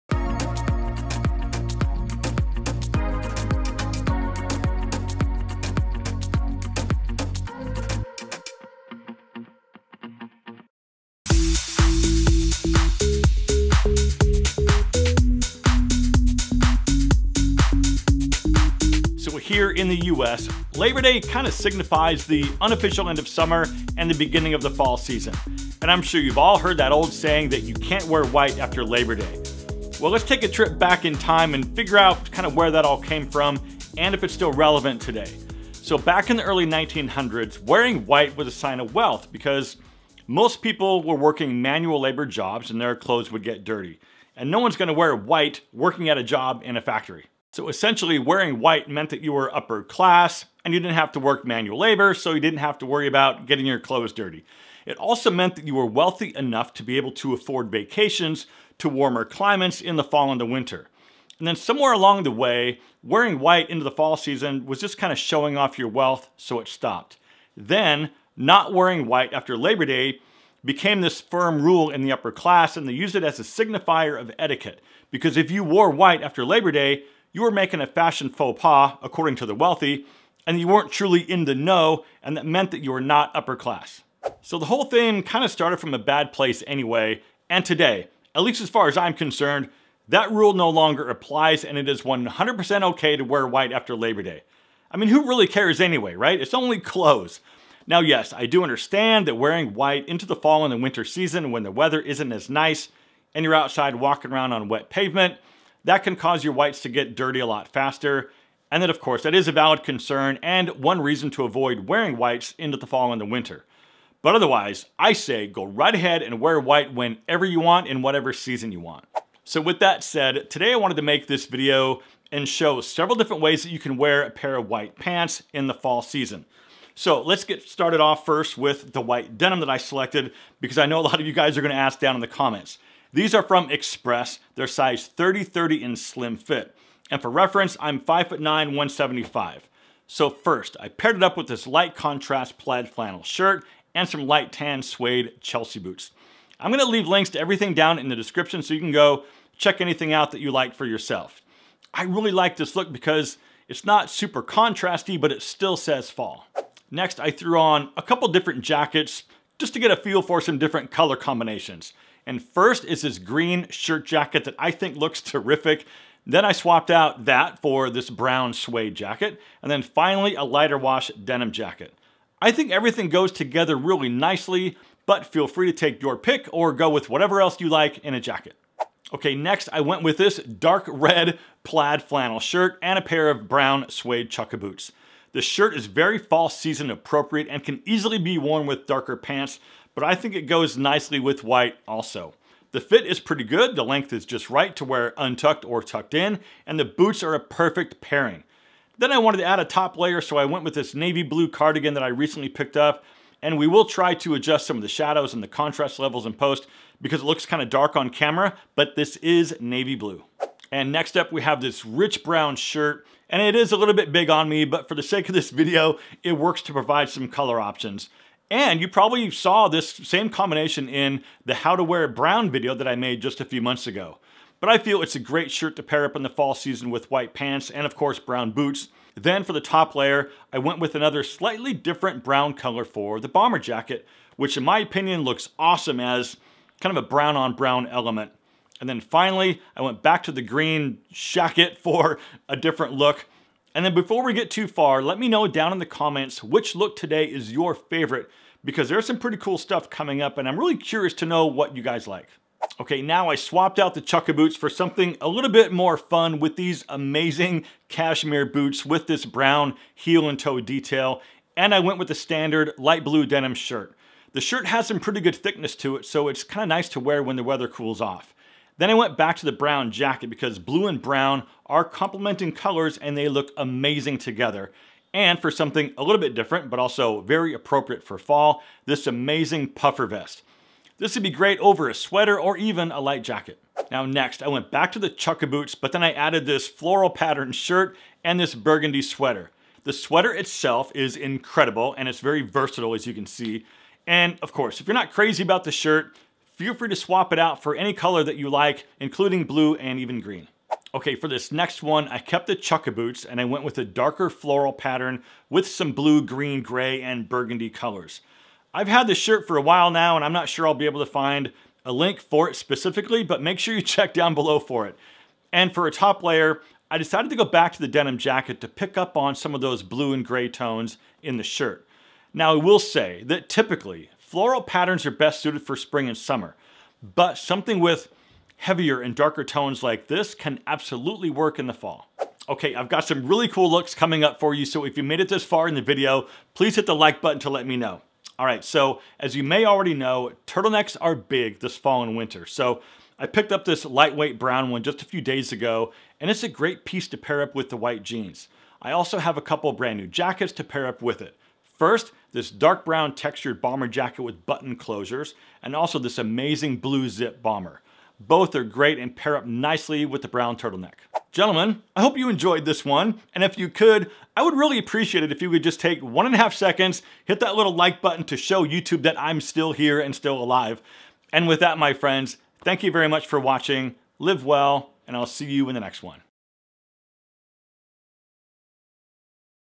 Here's HOW To Wear WHITE After Labor Day Over 40
Today I'm showing several different ways you can wear and style white pants in the Fall and Winter.

Red Flannel Shirt
White Denim
Cream Plaid Flannel
Floral Button Down (Similar)
Brown Turtleneck
Brown Plaid Bomber
Blue Plaid Bomber

\\ My New 1-on-1 Wardrobe Course!!

\\ 40 Over Fashion AMAZON STORE!